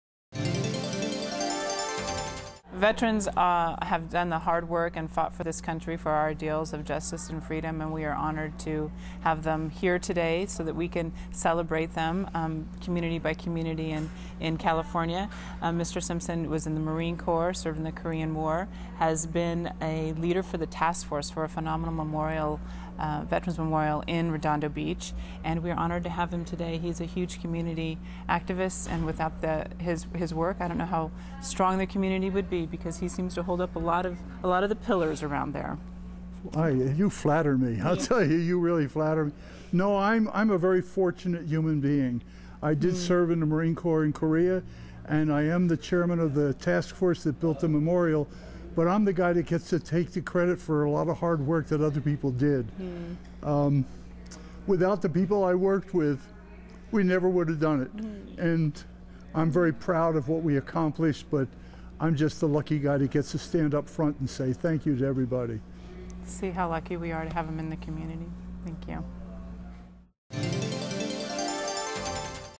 Assemblymember Butler Honors Marine Corps Veteran, Community Activist
(Sacramento) -- John Simpson is a veteran of the U.S. Marine Corp. After serving his country in the Korean War, Simpson came home to serve the veteran community in Redondo Beach.  California State Assemblymember Betsy Butler (D-Marina Del Rey) honored Simpson as Veteran of the Year for Assembly District 53.